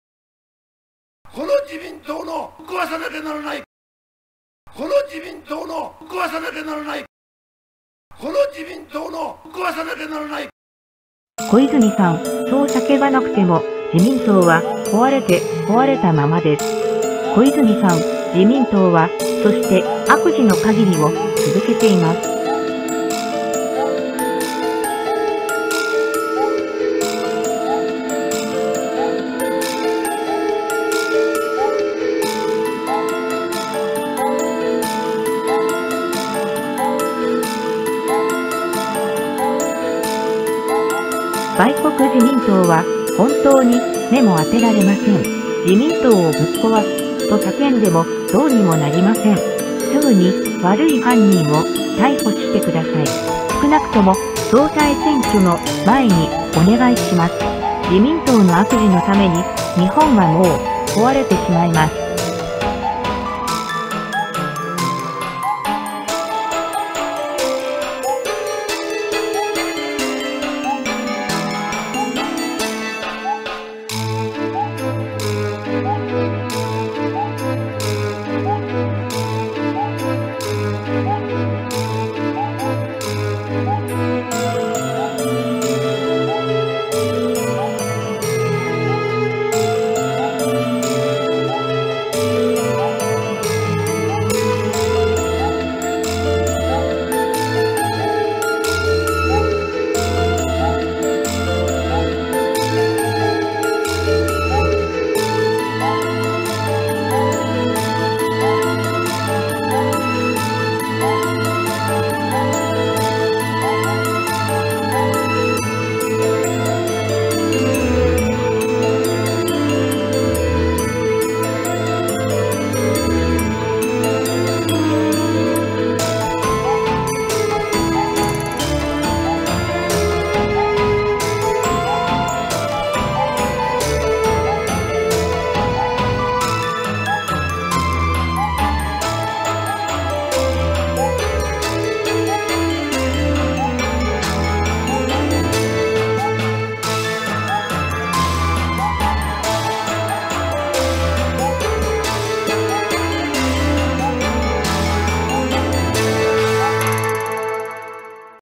小泉さん,"自民盗ぶっ壊す"言わなくても,壊れて,悪事が止まりません,総裁選挙前に悪人を逮捕して下さい。逮捕、お願いします。
引用：自民盗の悪事アーカイブを取り入れました。